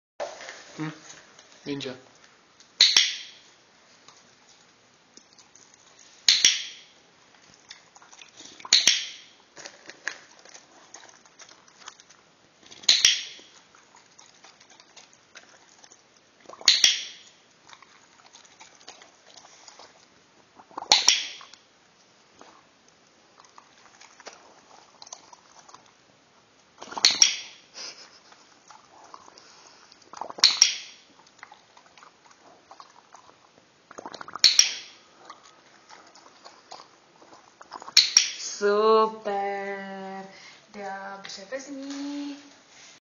Max- pouštění bublinek do vody
Maxík pouští bublinky do vody